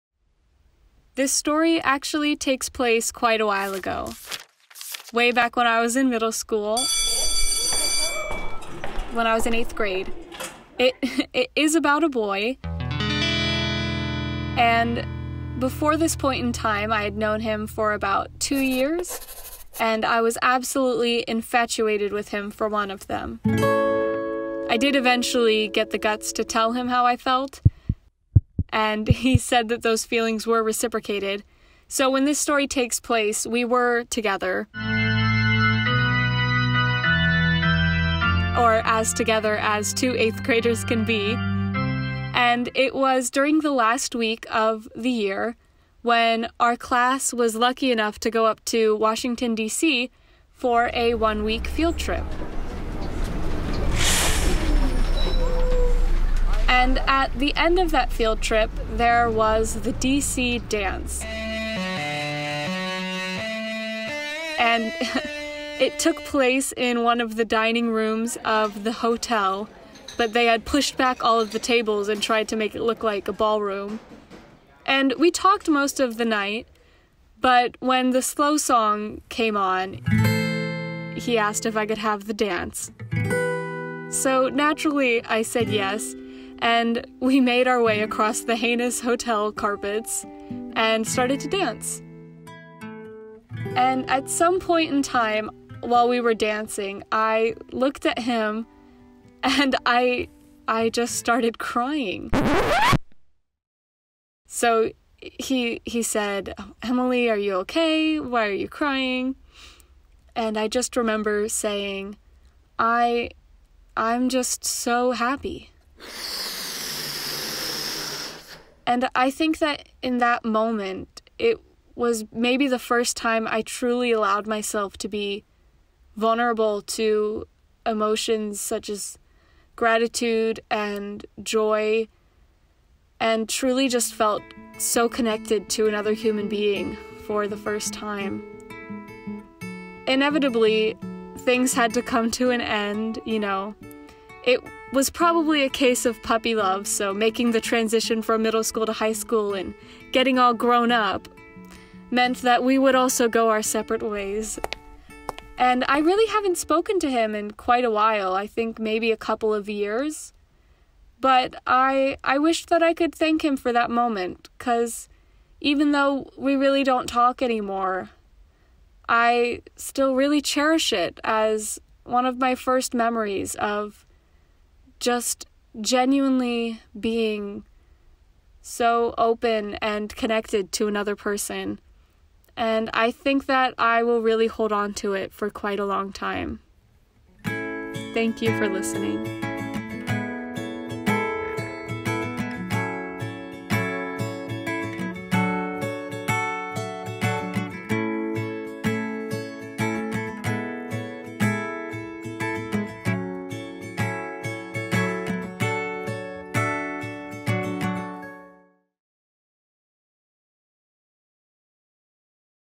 Unmasked: "Puppy Love" — Emily Miller
Emily Miller, a second-year pre-major in moving image production, danced with her 8th-grade crush on their Washington, D.C., field trip when all of a sudden she began to cry.